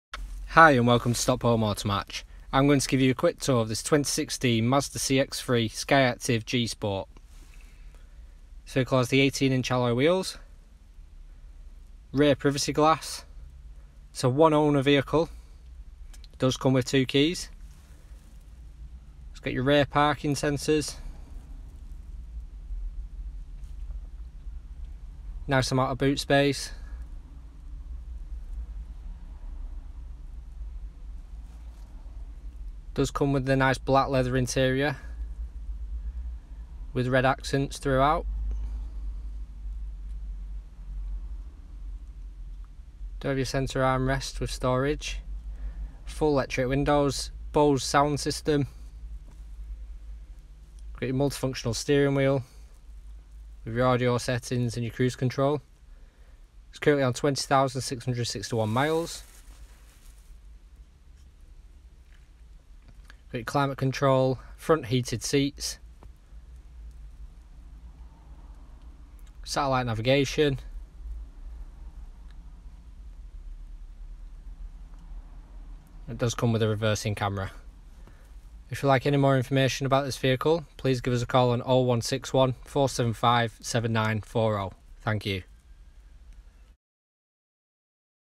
Mazda CX-3 | Stockport Motor Match
Check out this Mazda CX-3 2.0 SKYACTIV-G Sport which we have for sale at Stockport Motor Match.

KF66JFK
20,649 miles
1 owner
Petrol
Manual

Features include:

Satellite Navigation
Heads up Display
BOSE Sound System
Reversing Camera
Rear Sensors